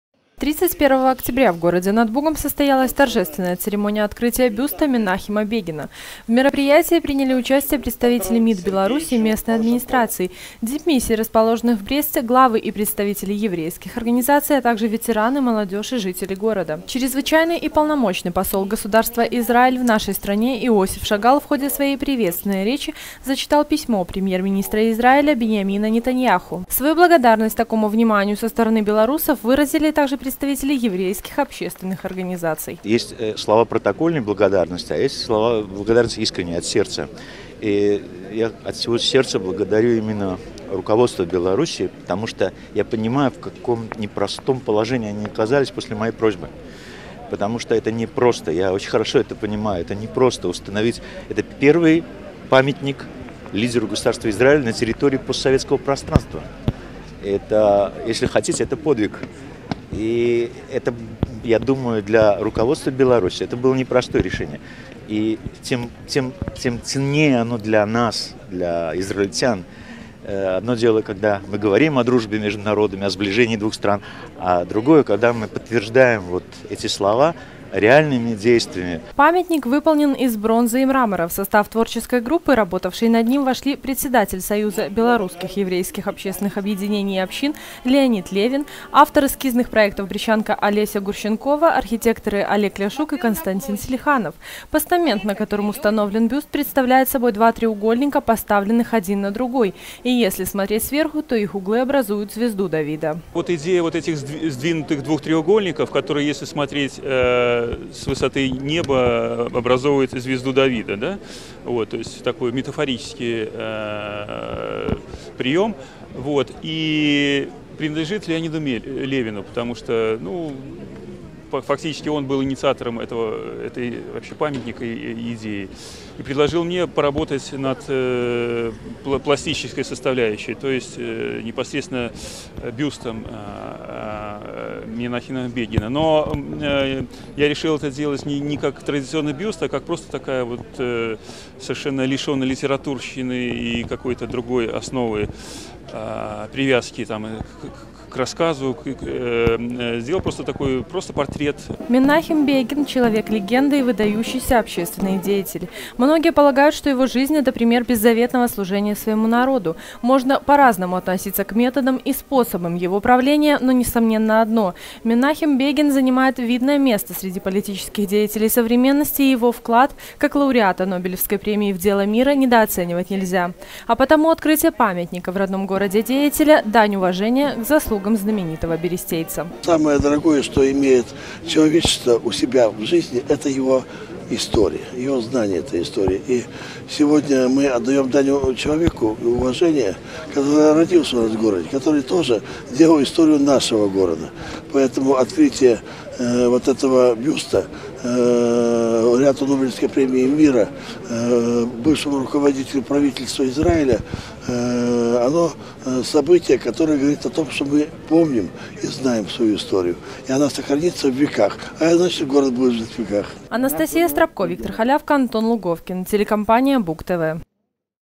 2013-10-31 г. Брест Телекомпания "Буг-ТВ". Торжественное открытие в г. Бресте бюста МенахемаБегина.
Ровно век назад родился МенАхемБЕгин, седьмой премьер-министр Израиля и лауреат Нобелевской премии мира. В городе над Бугом, месте,  где родился  политический деятель, в эти дни проходят памятные мероприятия. Сегодня состоялась торжественная церемония открытия бюста, установленного неподалёку от дома, в котором находилась школа, где учился БЕгин. Наша съёмочная группа побывала на открытии.